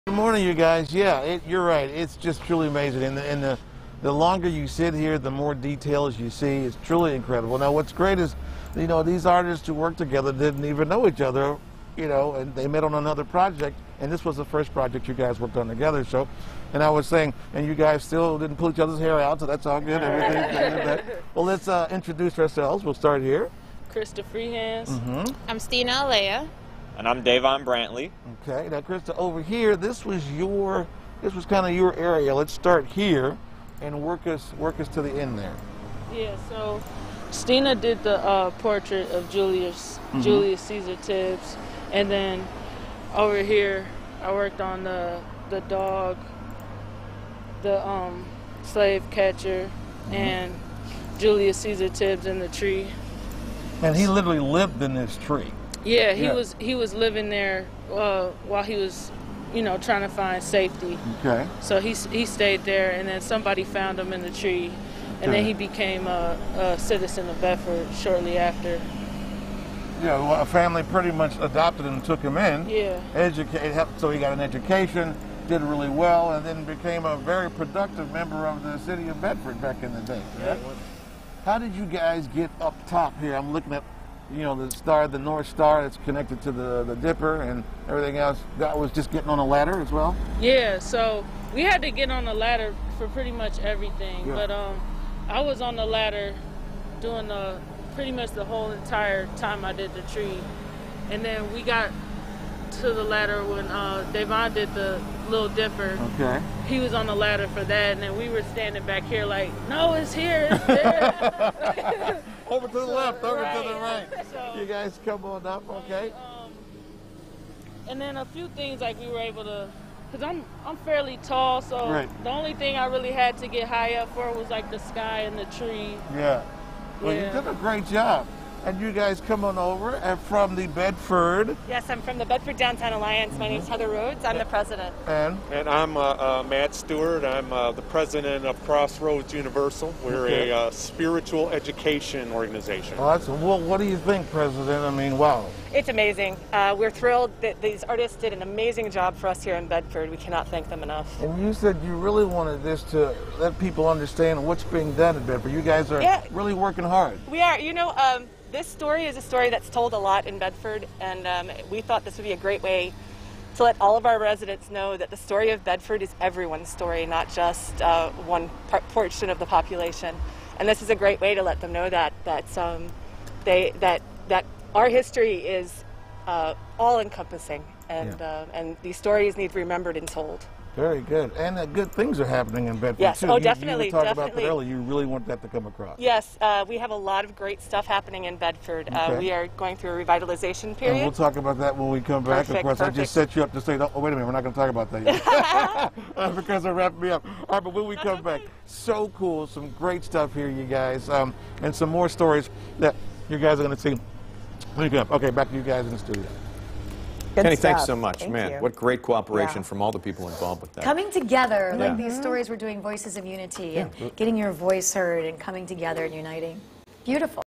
City of Bedford's role in the Underground Railroad illustrated in new downtown mural
The city of Bedford played an important role in the Underground Railroad and a recently completed mural illustrates that history. Fox 8's Kenny Crumpton spent the morning with the three talented artists who were commissioned to create the detailed artwork. He also learned how different parts of the mural depict different pieces of Bedford's history helping people with the Underground Railroad. To learn more about this beautiful 'Road to Freedom' mural